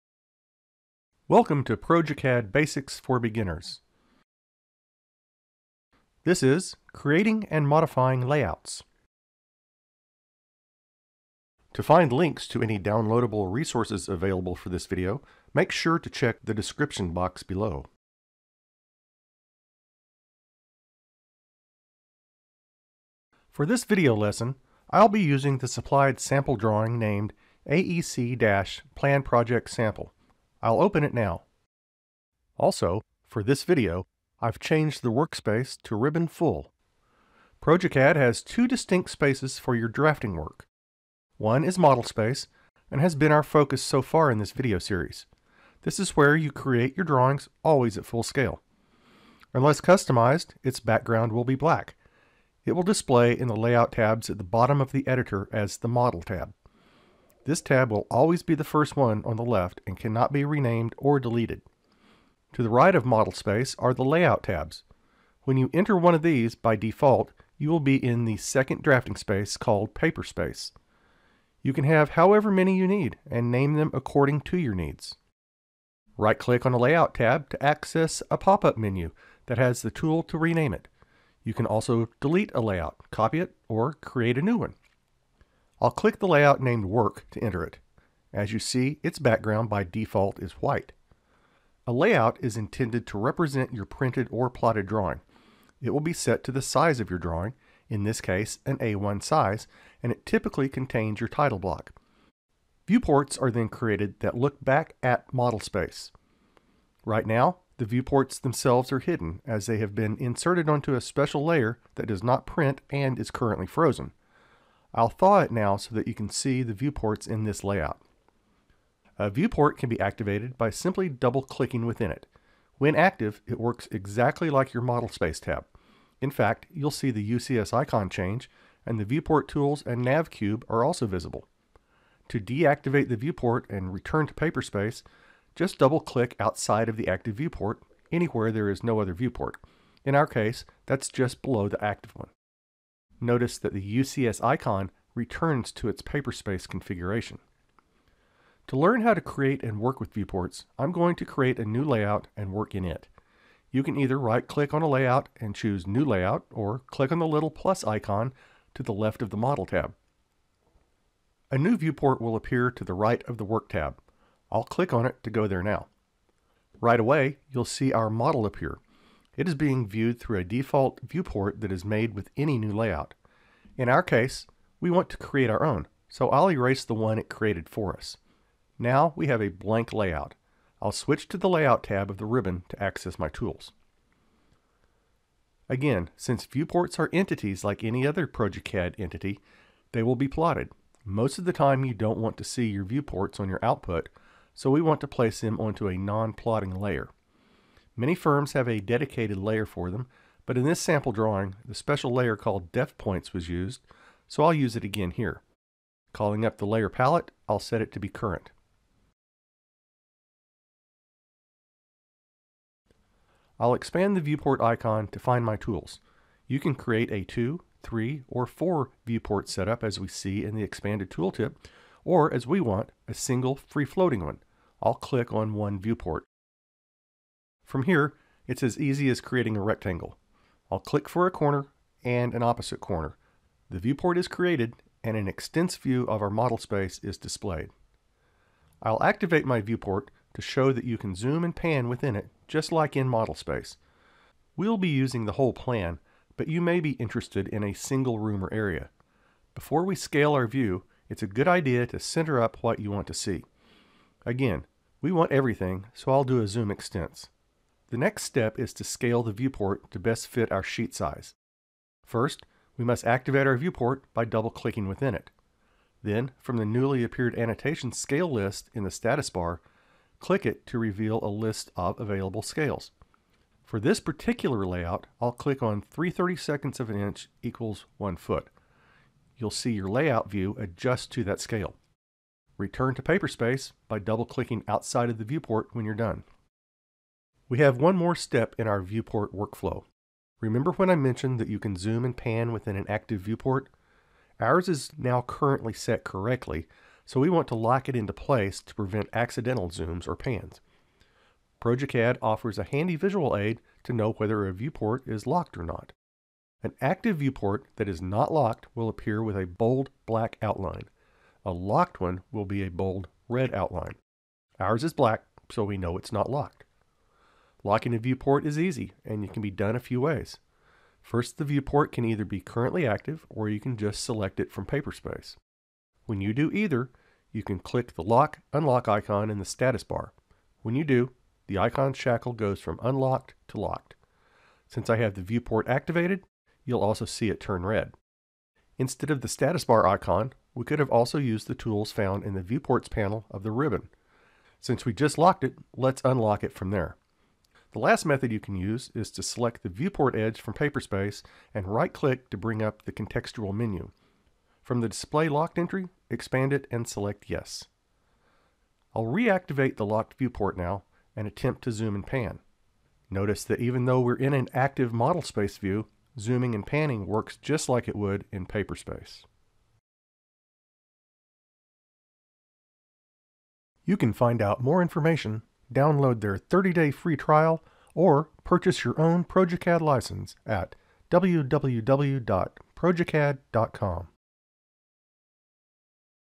1.7 - progeCAD Basics for Beginners - Creating and Modifying Layouts
progeCAD Tutorial 📽️In this video, we’ll talk about creating and modifying layouts in progeCAD. You'll learn the difference between Model Space and Paper Space, how to create and manage viewports, and how to work effectively between different spaces. Understanding layouts and viewports is essential for preparing your drawings for printing and presentation.
❓After watching the video, take the quiz to check your understanding! 🗂️Click links below to download the support materials:

00:00 Introduction
00:40 Model Space and Paper Space
01:05 Managing Layout Tabs
01:43 Viewing and Activating Viewports
02:29 Creating a New Layout
03:35 Creating a Viewport
04:00 Navigating Within a Viewport
04:18 Scaling a Viewport
04:48 Locking and Unlocking Viewports

progeCAD Professional, the 2D/3D DWG/DXF-native CAD platform, the Real CAD alternative.
AutoCAD, BricsCAD, ZwCAD, GstarCAD replacement with perpetual license. No annual fee.